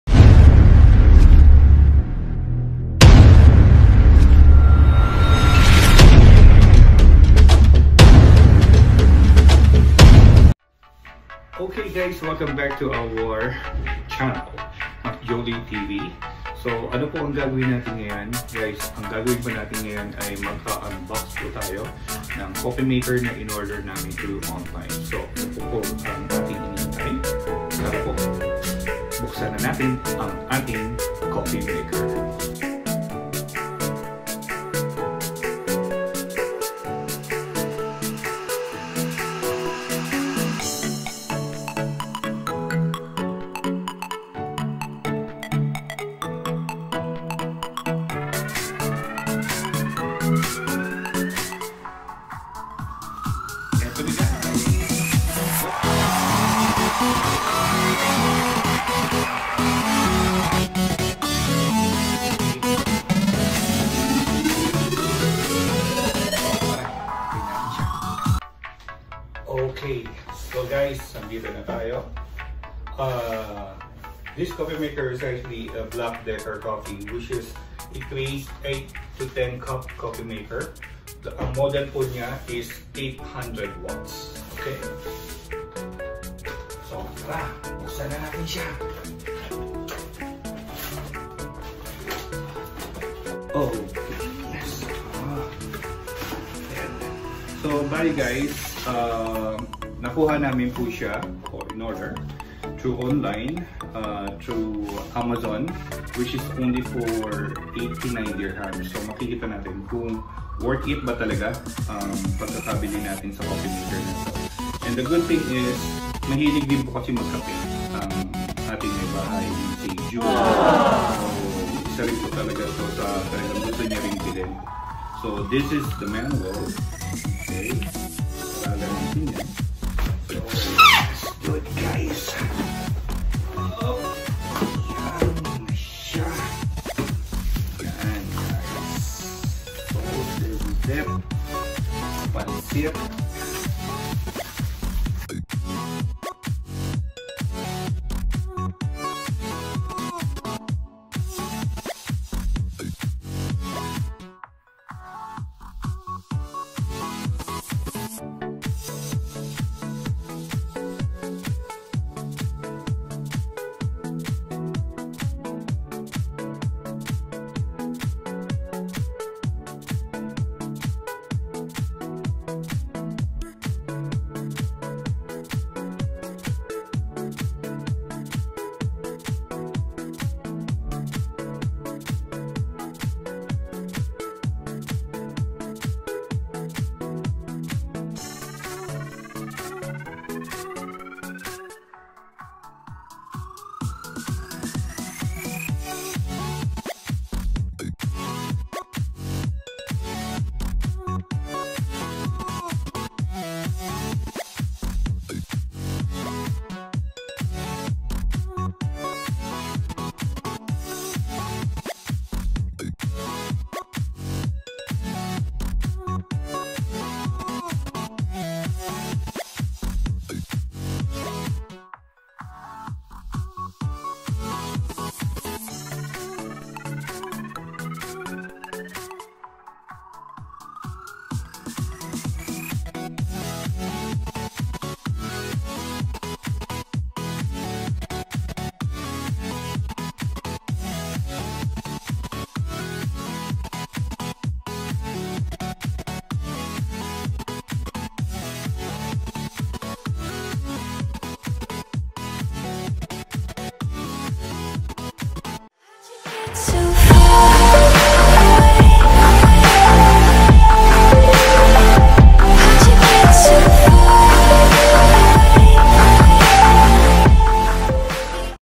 Unboxing Ang Inorder Namin sa (Amazon)Black Decker Coffee Maker Video Vlog # 5
This is Our Video Vlog # 5 This Video are Unboxing Our Ordered Coffee Maker Online.Since Wifee is So Mahilig on Coffee matagal nadin pong Plan namin kumuha nito..Hope you enjoy Watching Our Video Please dont forget to Like and Subscribe...